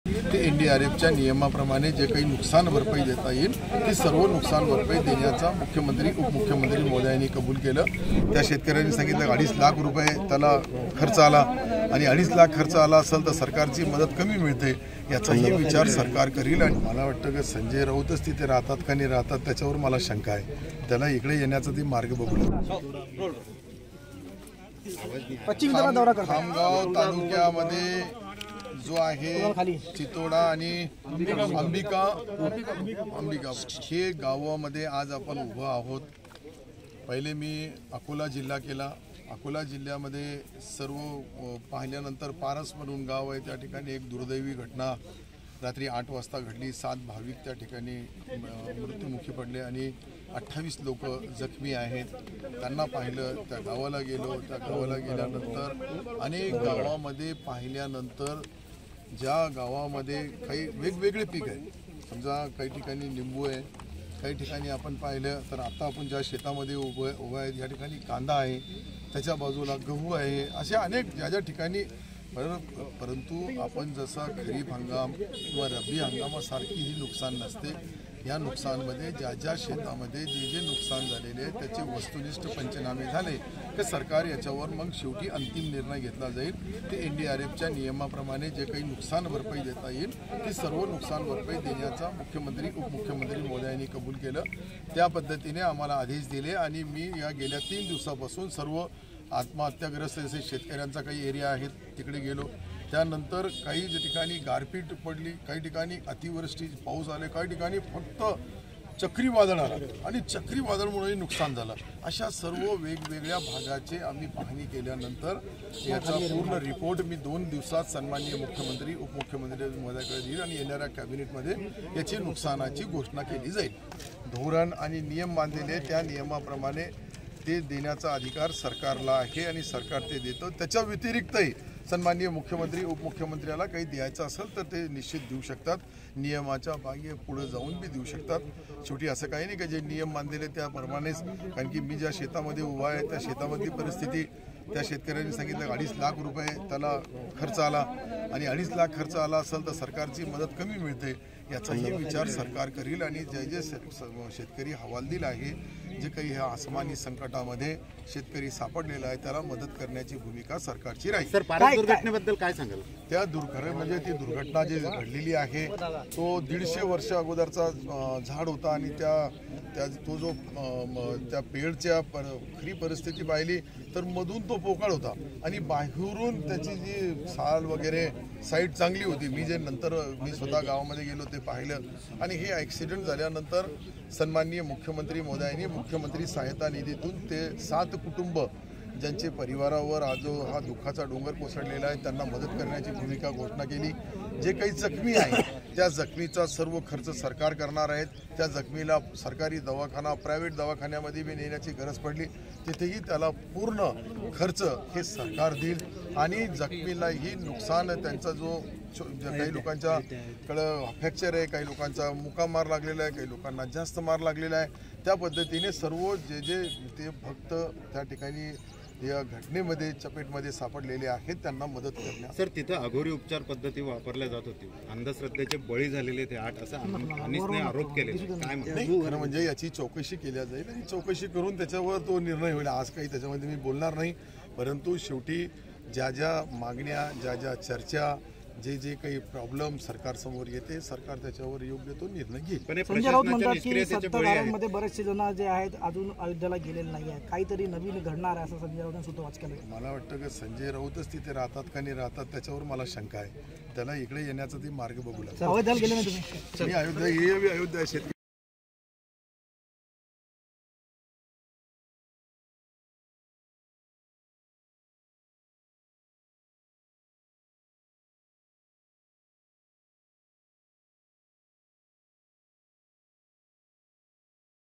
Abdul Sattar on Sanjay Raut : संजय राऊतांचा दावा सत्तारांनी खोडून काढला... | LetsUpp Marathi
कृषीमंत्री अब्दुल सत्तार यांनी नुकसानग्रस्त भागांची पाहणी केली. राज्य सरकार शेतकऱ्यांच्या पाठीशी खंबीरपणे उभे आहे असा विश्वास कृषिमंत्र्यांनी यावेळी उपस्थित शेतकऱ्यांना दिला.